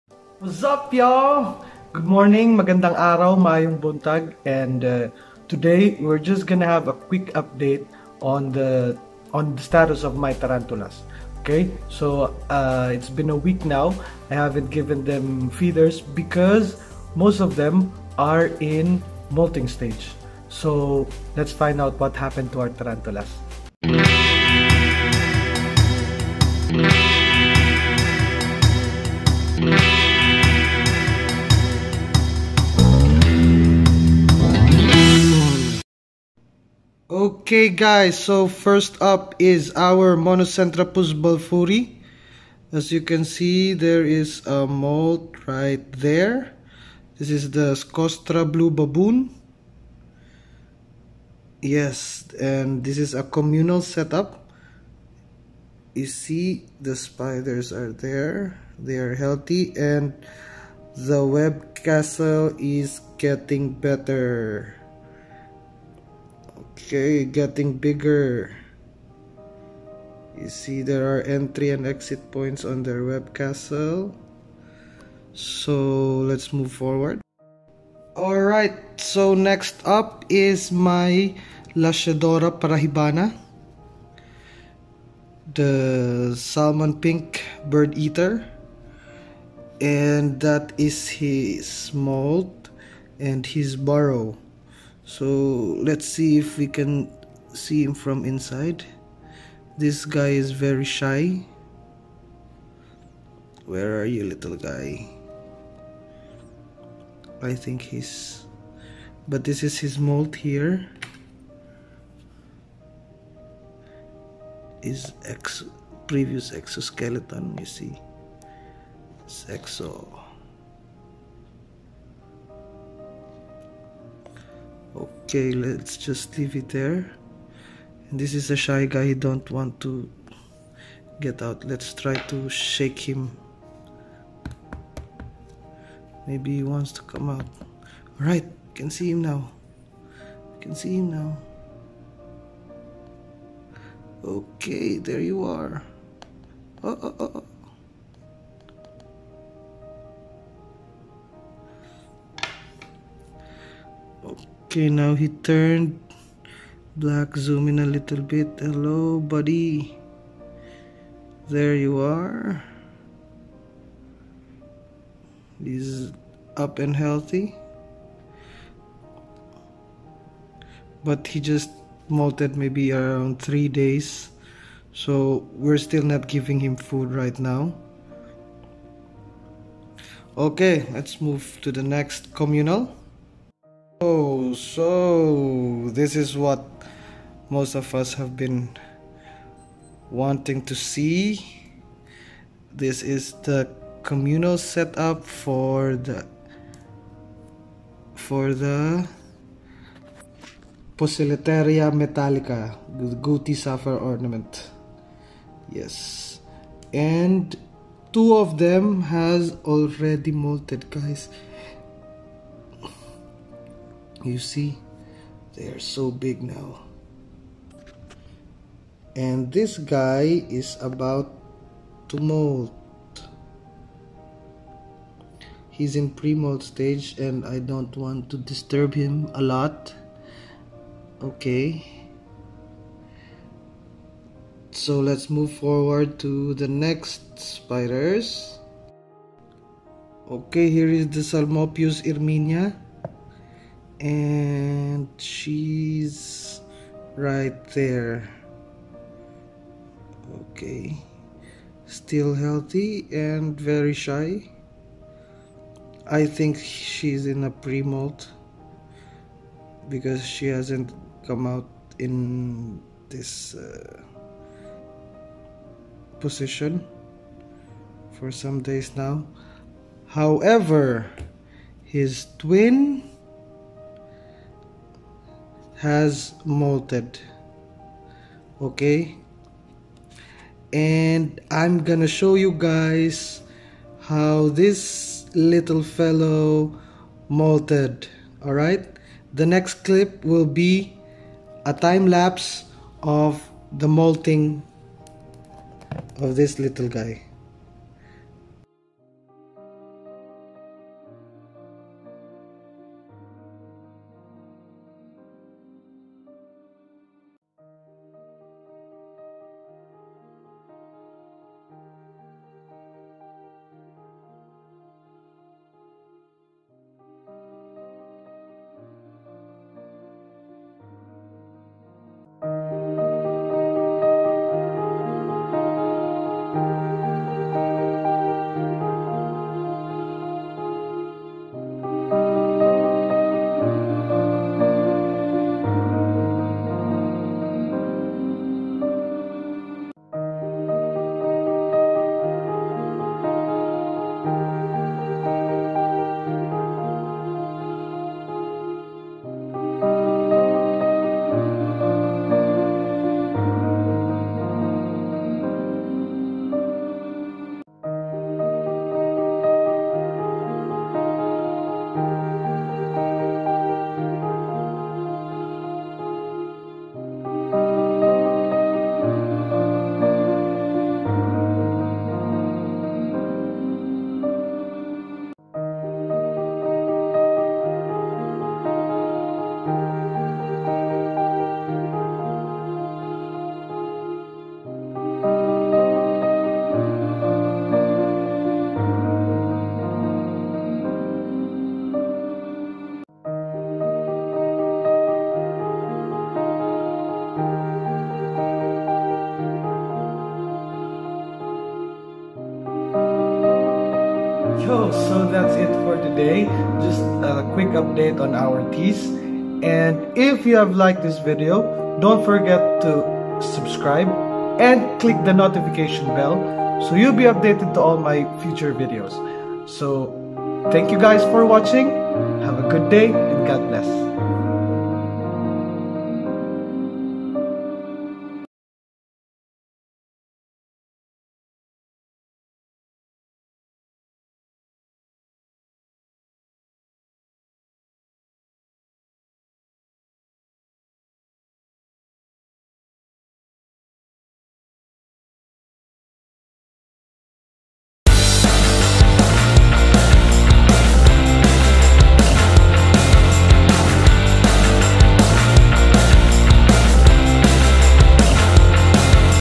Tarantula Update | Spiderling Molting
Vlog#7: Vince has given an update about his tarantulas where most of them are in pre-molt stage. He will also show a time lapse of a spiderling molting.